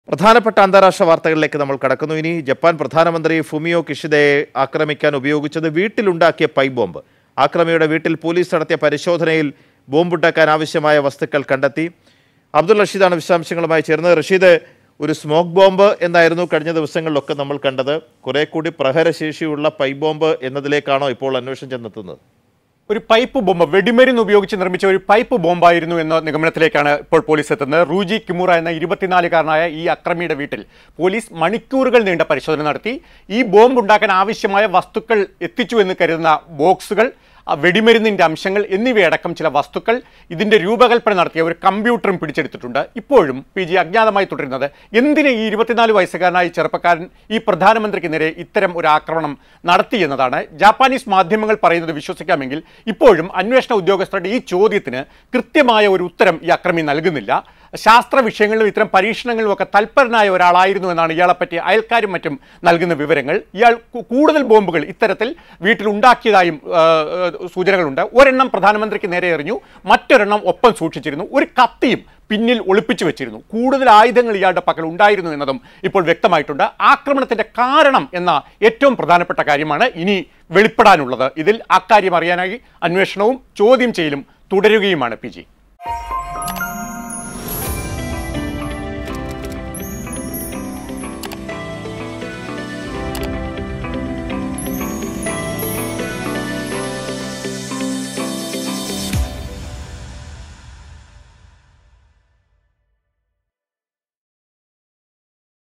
ജപ്പാൻ പ്രധാനമന്ത്രിയെ ആക്രമിക്കാൻ ഉപയോഗിച്ചത് പൈപ്പ് ബോംബ്| Japanese PM attacked
Japanese PM attacked updates
ജപ്പാൻ പ്രധാനമന്ത്രിയെ ആക്രമിക്കാൻ ഉപയോഗിച്ചത് പൈപ്പ് ബോംബ്; അക്രമിയുടെ വീട്ടിൽ നിന്ന് ബോംബ് ഉണ്ടാക്കാൻ ആവശ്യമായ വസ്തുക്കൾ കണ്ടെത്തി